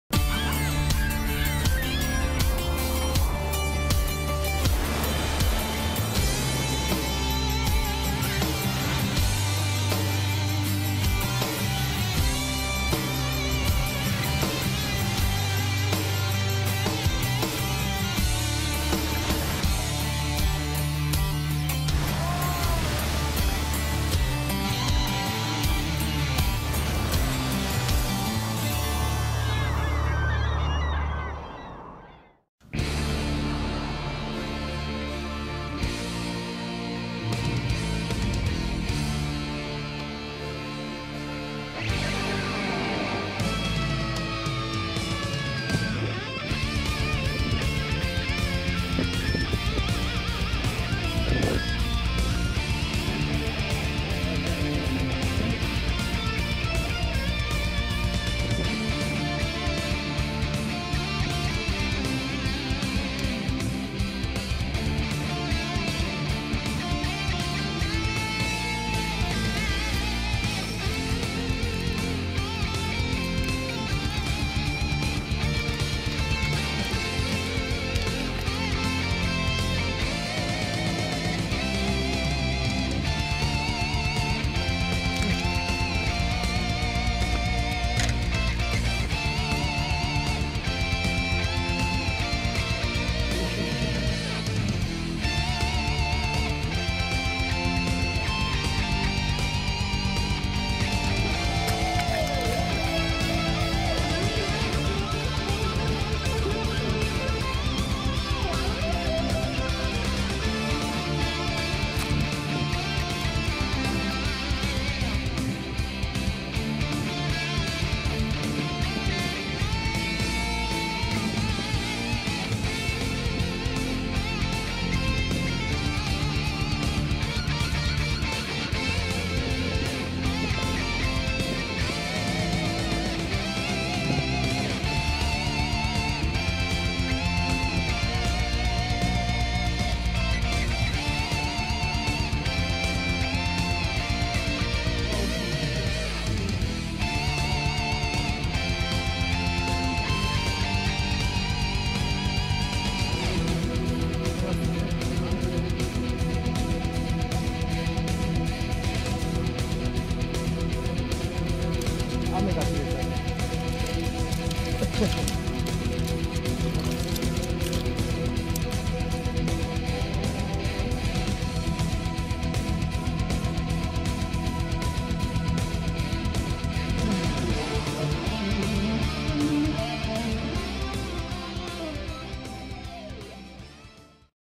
2022年ホッケ釣りも終盤!! 西積丹の磯で大型ホッケを仕留めろ!!
今シーズンラストの可能性がある春ホッケ釣り。